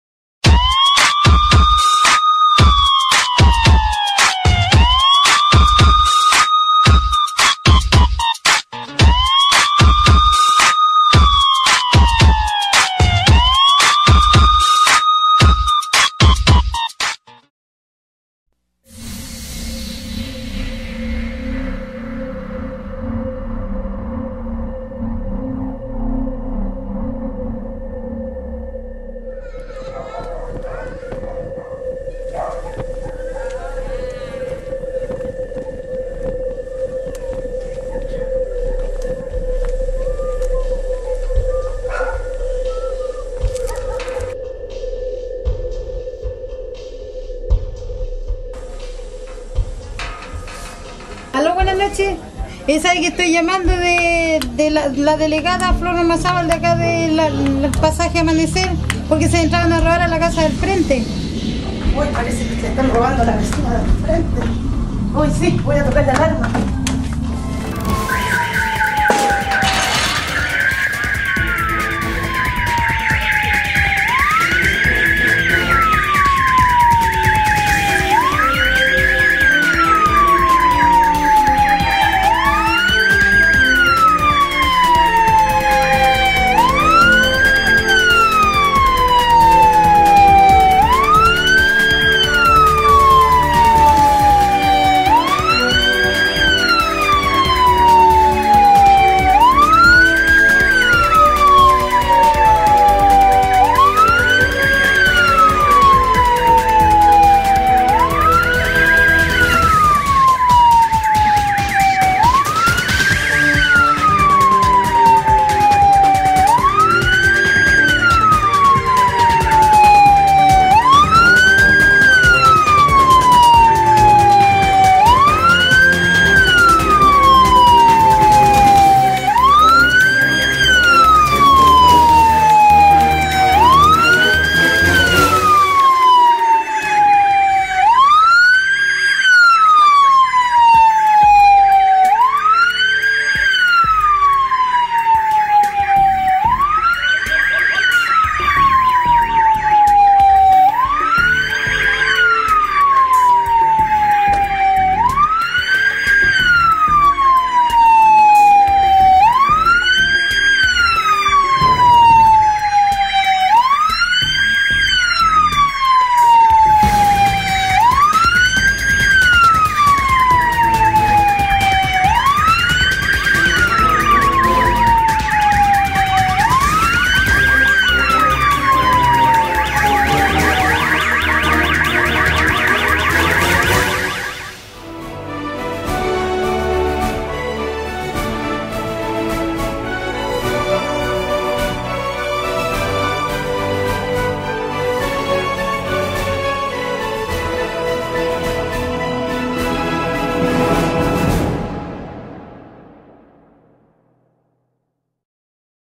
Simulacro Alarmas Comunitarias 2
Simulacro desarrollado en la ciudad de Linares, por medio de la Municipalidad y su programa Seguridad Pública-Alarmas Comunitarias.